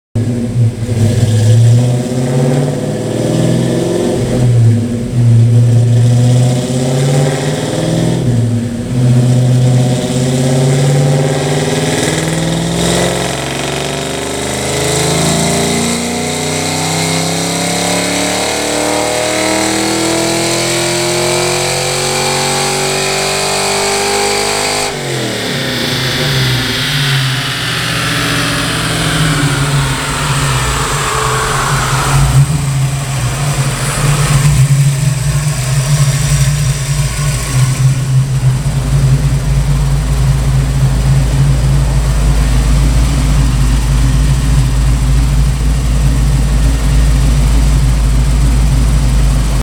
(87-93) 1990 Mustang GT Dyno With New Trick Flow Top End 2.MP4
Trick Flow Top End Kit producing the results 293 RWHP and 300 Torque.  Dyno before build was 207 RWHP and 265 Torque.  See our other videos for the "before" dyno and some other sweet mustang footage.